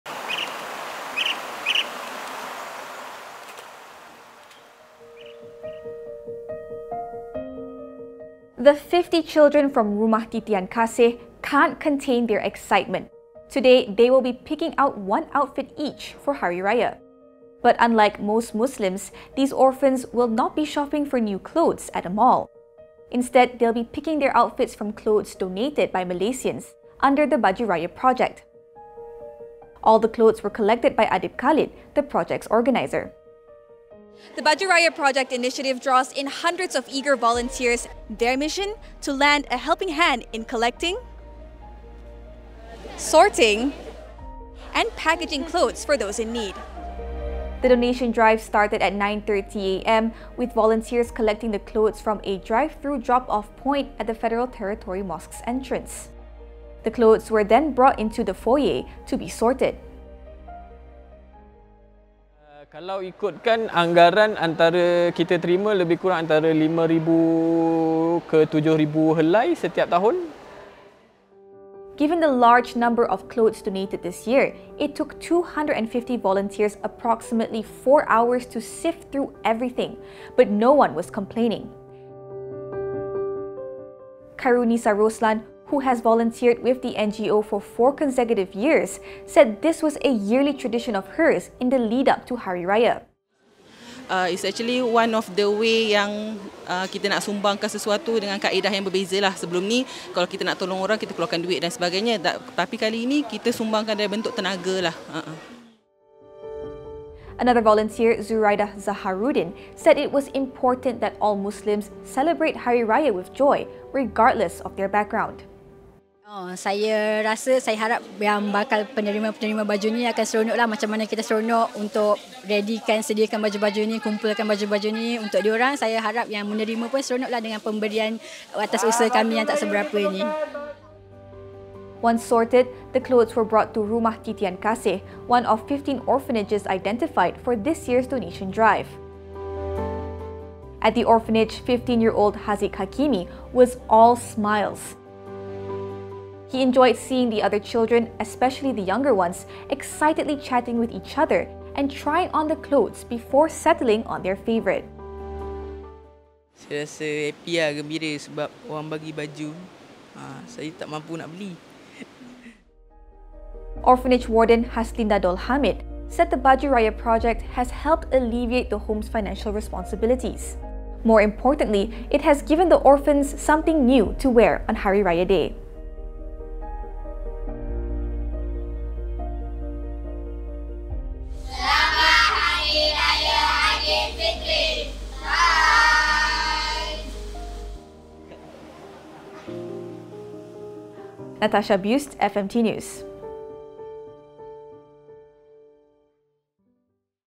Spreading Raya joy through acts of kindness
Generous Malaysians volunteer their time for the Baju Raya Project initiative, proving that not only monetary donations count.